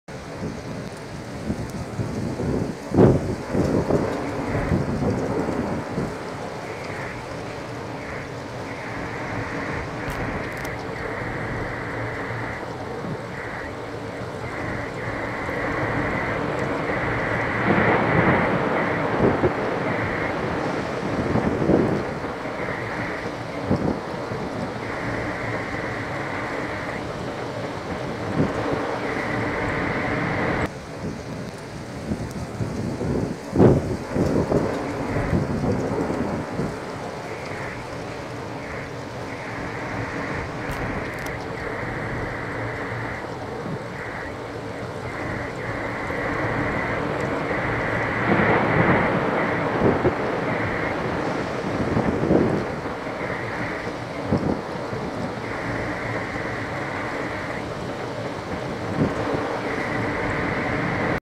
Cortland tornado
Tornado hits Cortland, Ohio.